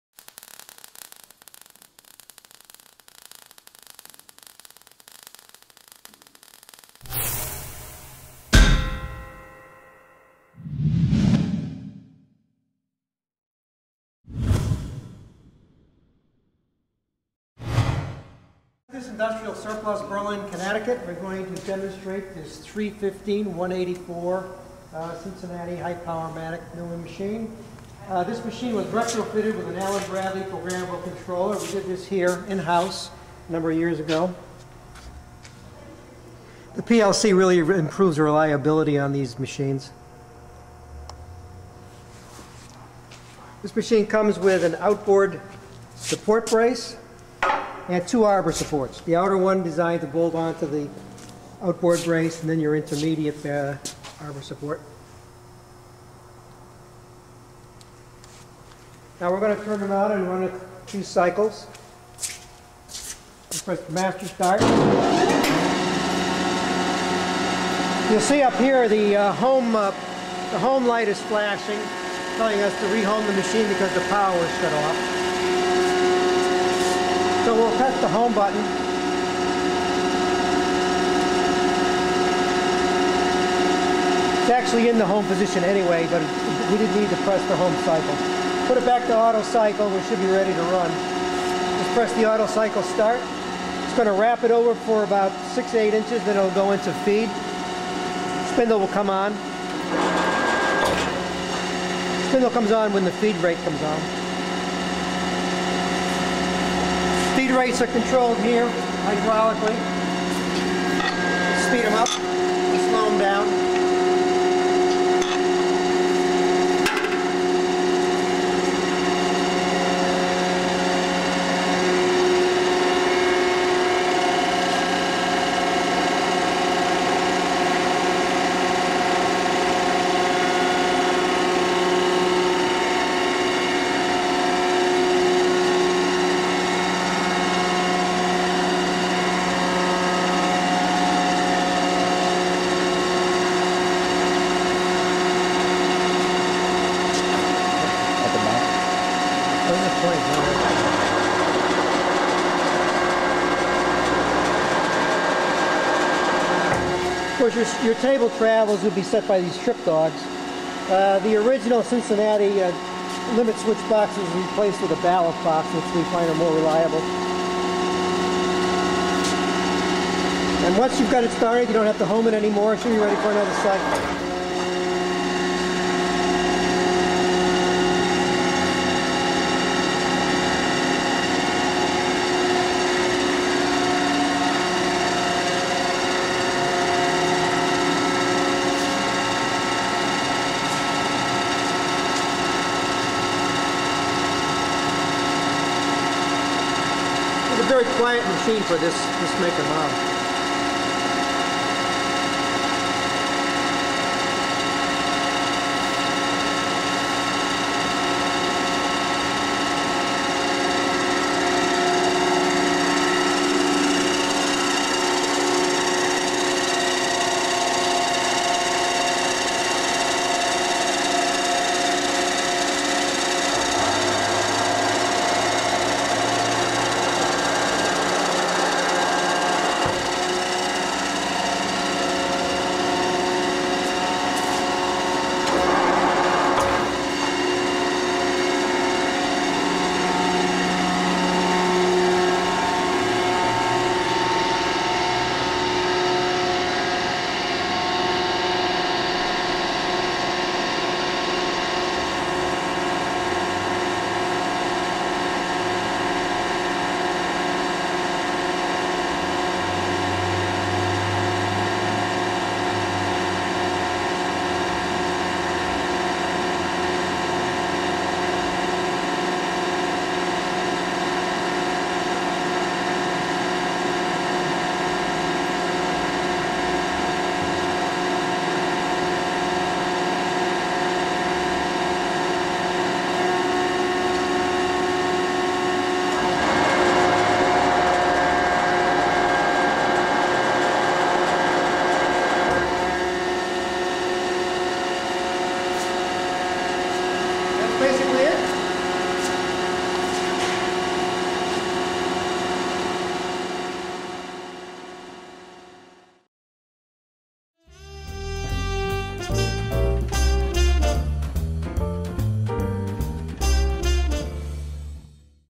Cincinnati Horizontal Production Mill, #315-184 Pl. Hypowermatic, Ref.#76-1082 - SOLD
Cincinnati Horizontal Production Mill  /  Ref.#76-1082
Model No. 315-184  Plain Hypowermatic
Serial No. 311431P5V-6     New 1970

Retrofited with Allen-Bradley SLC 5/02 Programmable Controller.

Table Size:  18" x 72"
Table Travel:  48"
Vertical Travel of Head:  14"
Max. Distance Spindle Center to Table Top:  18.25"
Cross Quill Adjustment:  4"
Spindle Quill Diameter:  7.5"
Spindle Speeds:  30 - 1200 RPM
Table Feed Rates:  1 - 150 IPM
Machine Weight, approx.:  13,800 lbs.

Equipped With:
Outboard Support Brace
Speed Change Gears
Arbor Support
Coolant

15 HP Spindle 220/440/3/60
7.5 HP Hydraulic Motor